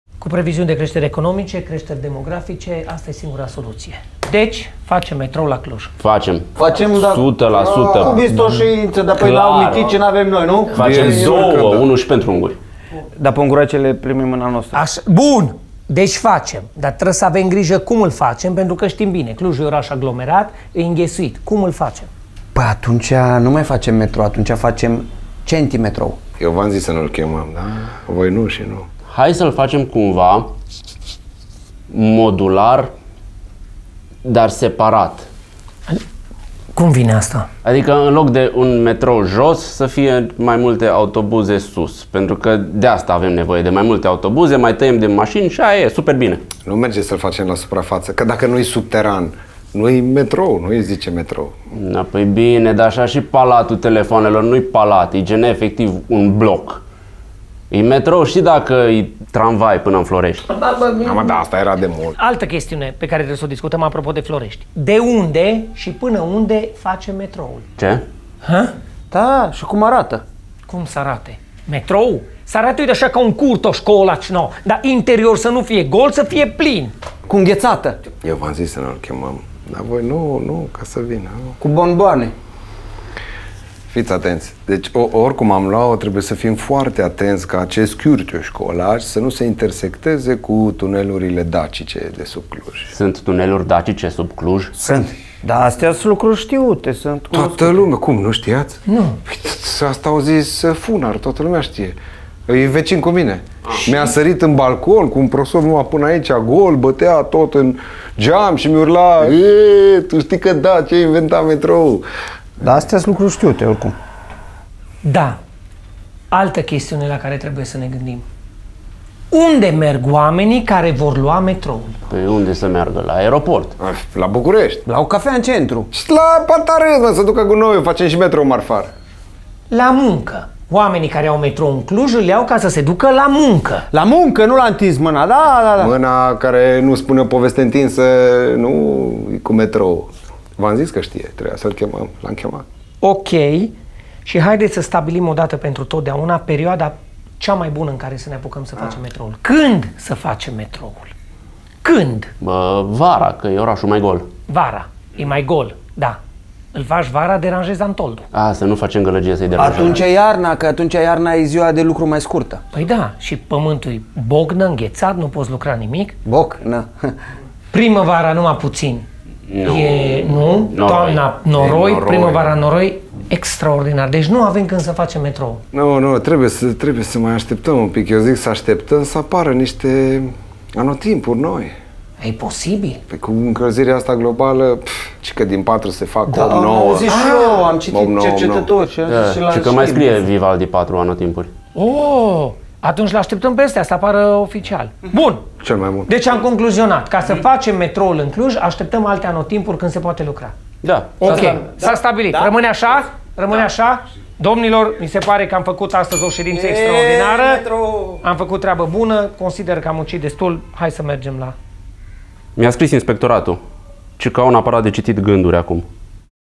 Se face metrou la Cluj | Epic Show
Se face metrou la Cluj. Aia e clar. În ce condiții se face el? Aia e mai puțin clar.

Au interpretat: Dragoș Stan, Bogdan Bob Rădulescu, Tibi Codorean, Adrian Cucu
A filmat, luminat, colorat, editat: Cezar 'Druidul' Enache
După un scenariu de: Tibi Codorean